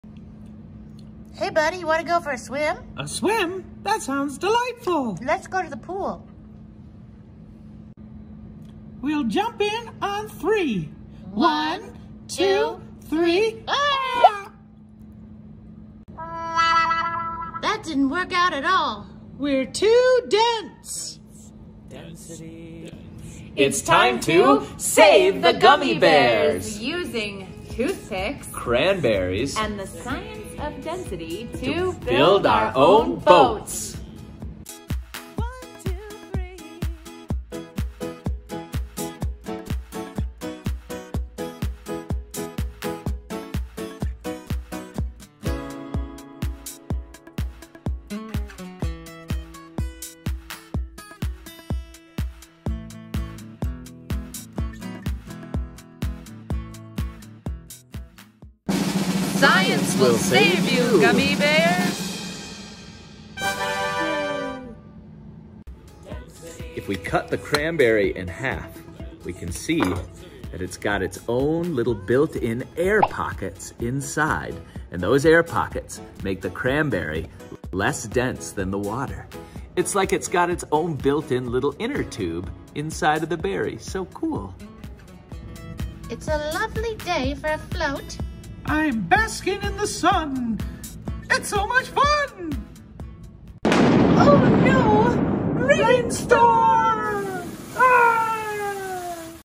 Save the Gummy Bears From The Green Goo - Kids Science Experiment - Air Pressure
Save the Gummy Bears a science experiment you can do at home!!

it's time to save the gummy bears we're using toothpicks cranberries and the science of density to build our own boats one two three